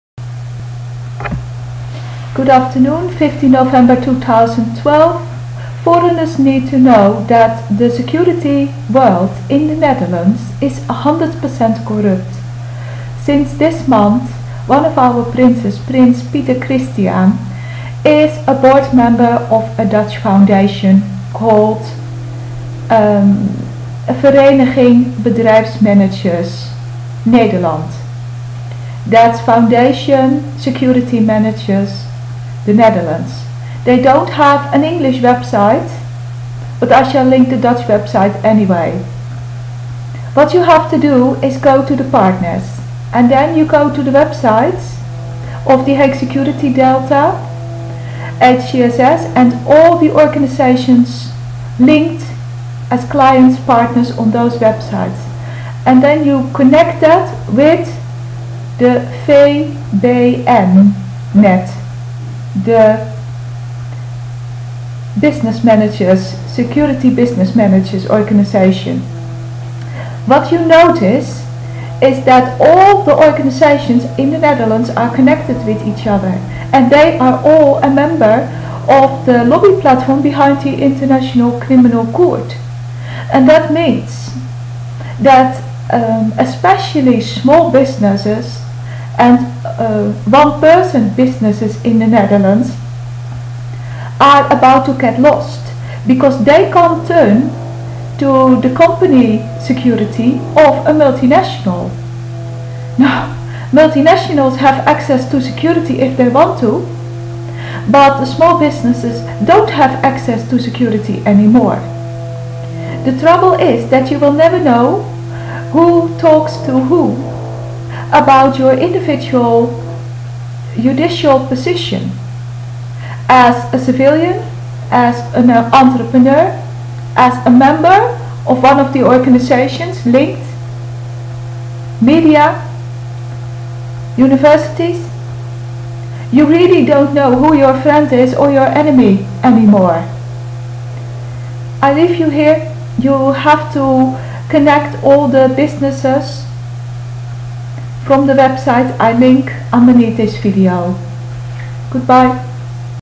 ICC.Royalfamily.corruption.Security.VBNnet.Elite.murder.platform.pedophiles.cybercrime.HCSS.Bank.wmv
PrinsPieterChristiaan works for VBNnet and happily participates in the ICC-lobbyplatform.

The Dutch RoyalFamily wants the breakdown of the National Secuirty in NL and in Europe. and Worldwide...

Individuals and foreigners have NO Judicial position left = the ICC-lobby decides WHO has acces to safety, security...justice... and who NOT.

Its most likely that the Dutch ROyal Family supports the making of a High-level Pedophile-club  behind the International Cybercrime Court.

All Tradeunions, Employers-organisation...etc... particpate in this ICC-lobby-world.

People must look nto the linked organisations for themselves, to find out HOW much danger they are in. individually.